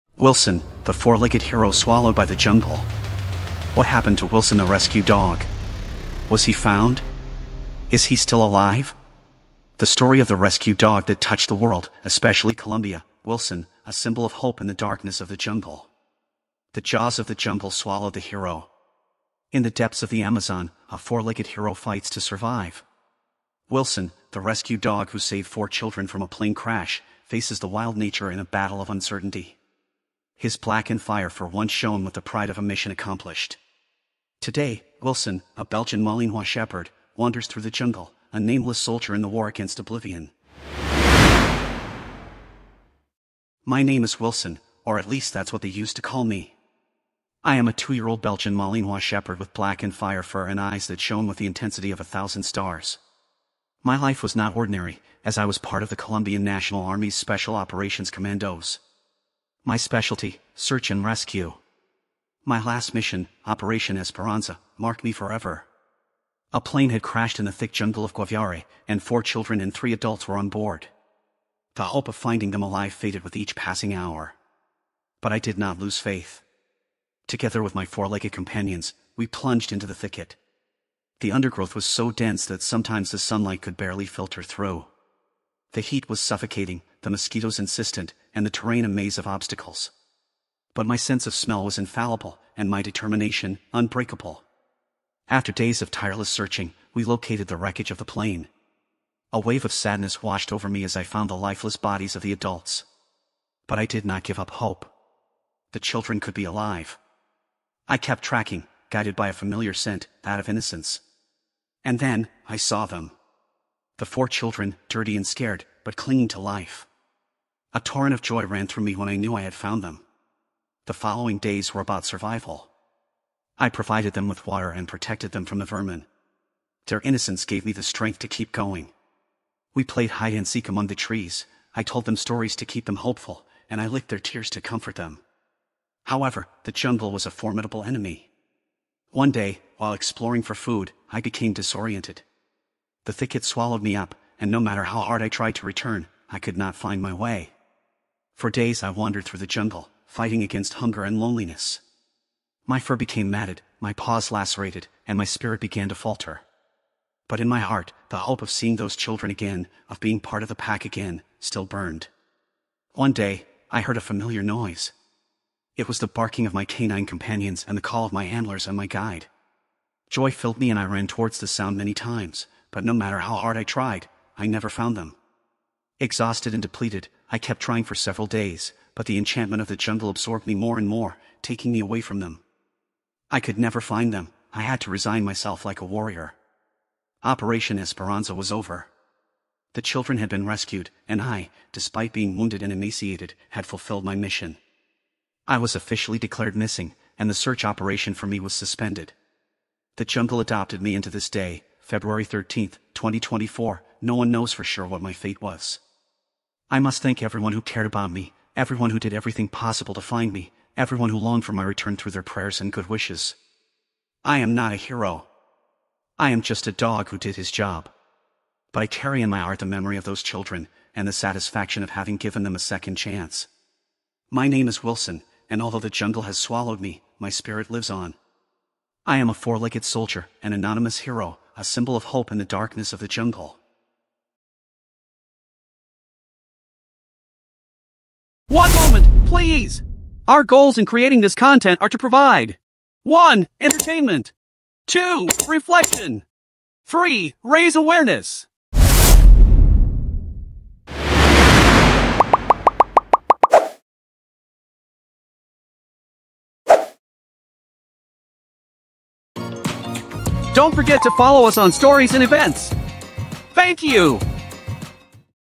Wilson: The Four-Legged Hero Swallowed by the Jungle
What happened to Wilson the rescue dog?
Was he found? Is he still alive? The story of the rescue dog that touched the world, especially Colombia, Wilson, a symbol of hope in the darkness of the jungle. The jaws of the jungle swallowed the hero.

In the depths of the Amazon, a four-legged hero fights to survive. Wilson, the rescue dog who saved four children from a plane crash, faces the wild nature in a battle of uncertainty.

His black and fire fur once shone with the pride of a mission accomplished. Today, Wilson, a Belgian Malinois shepherd, wanders through the jungle, a nameless soldier in the war against oblivion.